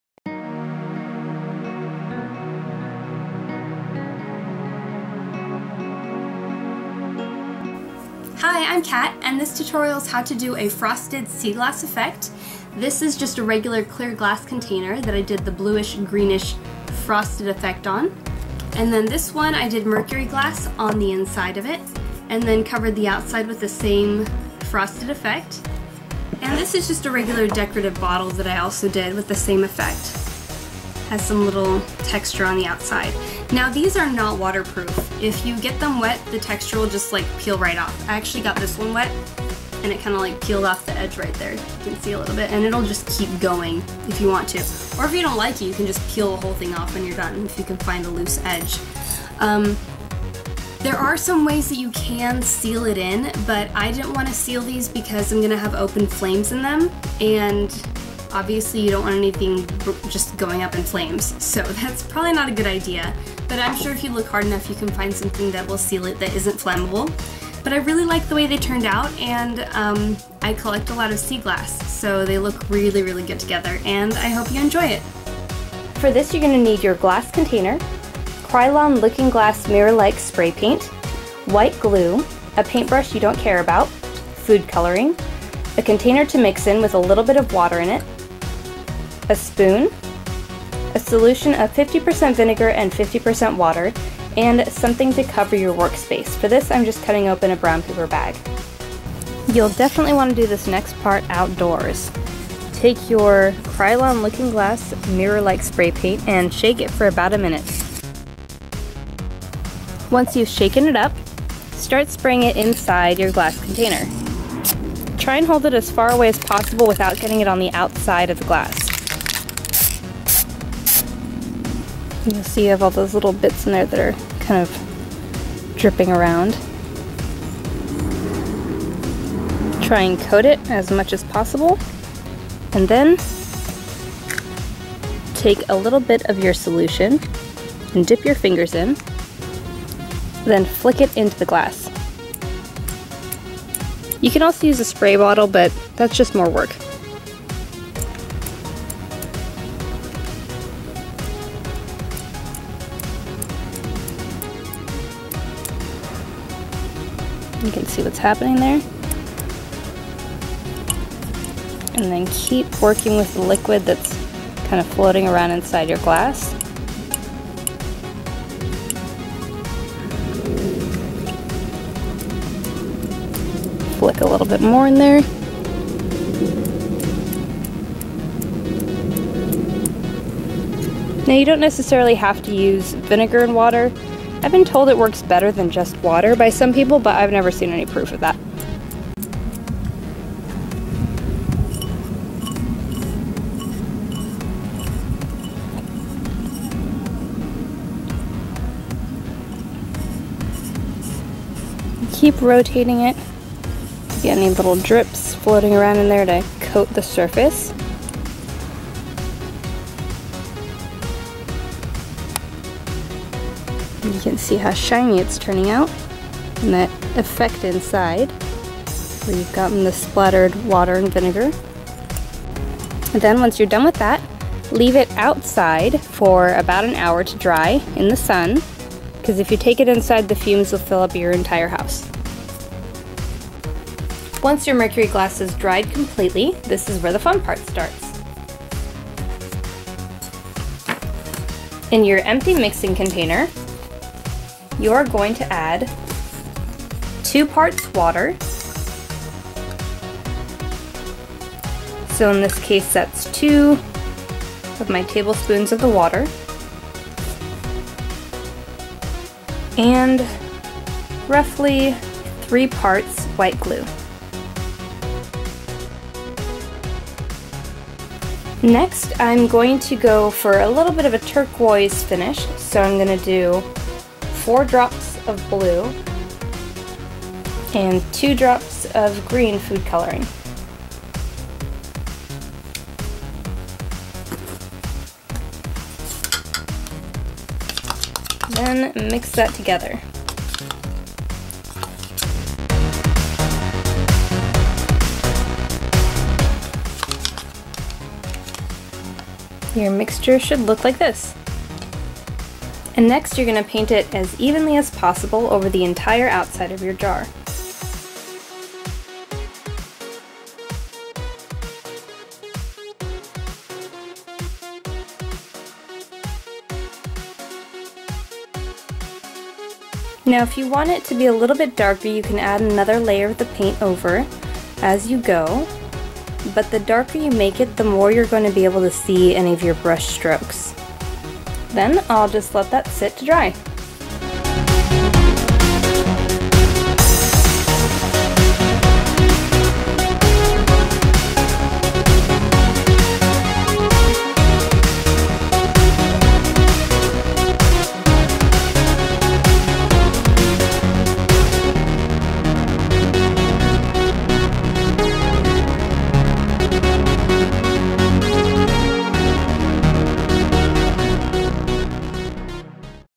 DIY Décor ♥ Frosted Sea Glass Candle Holders
How to turn glass, glue, and food coloring into something fabulous!

Follow Katrinaosity...

Mail:
Katrina Sherwood
90232